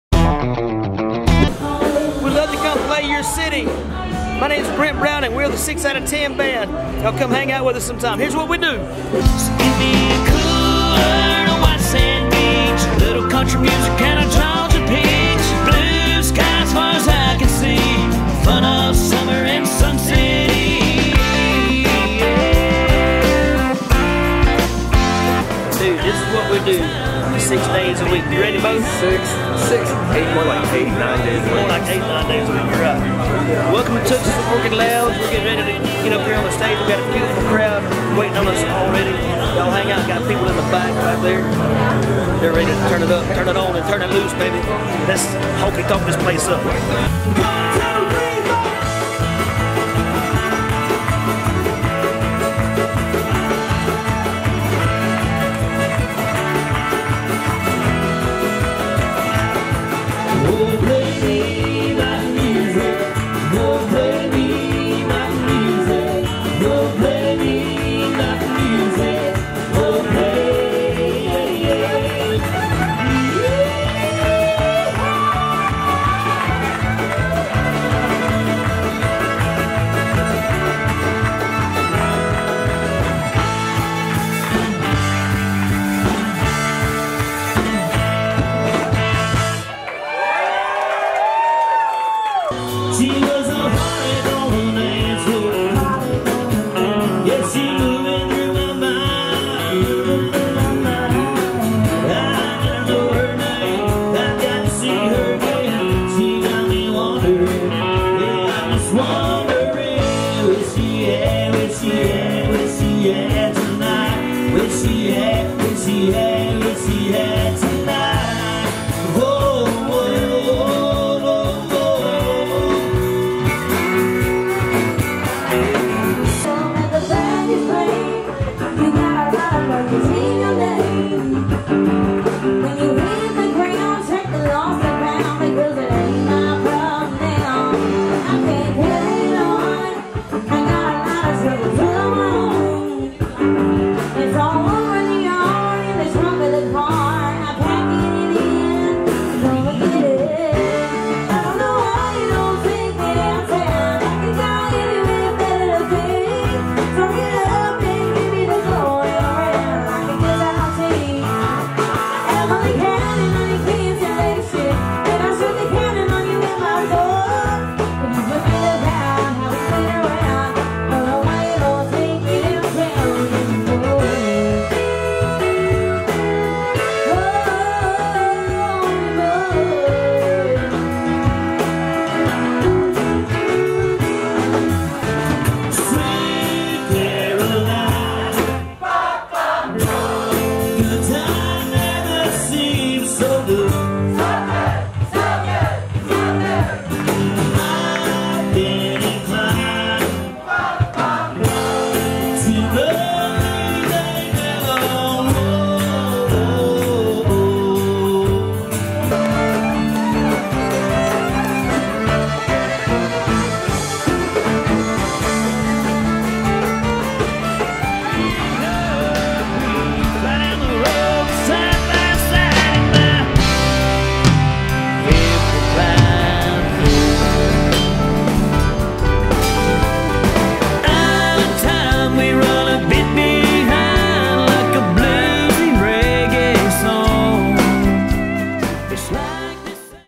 SIX OUT OF TENN BAND PROMO/EPK
Needing a band/entertainment for your venue, corporate event or pivate event? Take a look at our Band PROMO/EPK video for my band, the Six Out of Tenn Band! Booking info listed in the video!